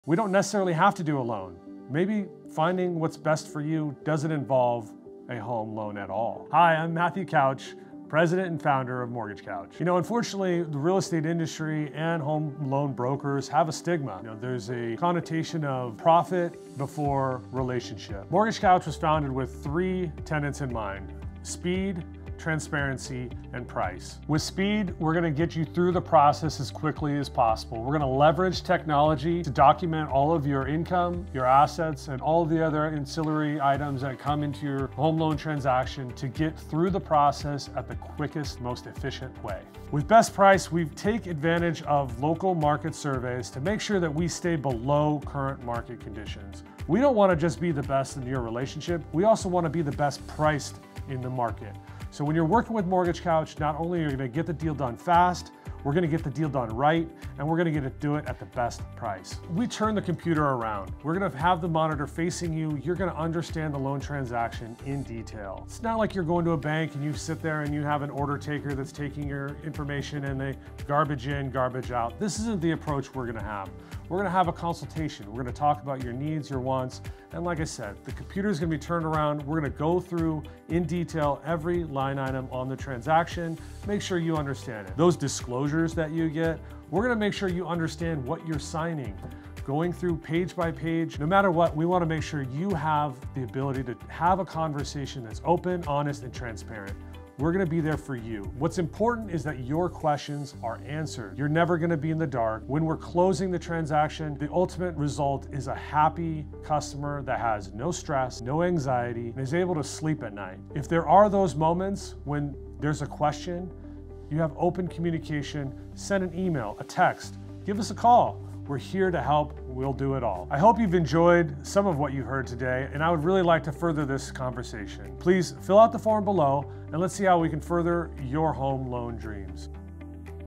MortgageCouch
An introduction of MortgageCouch by Matthew Couch, President and Founder since 2013.

MortgageCouch is celebrating 10 years of helping families in California, Oregon, and now Texas!

From manufactured homes in parks to estate homes for a thriving family, at MortgageCouch, we strive to identify the best wholesale lender for our clients. Whether looking for the best available terms or a specific lender to accommodate a unique situation, MortgageCouch has the resources and experience to meet every need.

MortgageCouch – Home Loans made Comfortable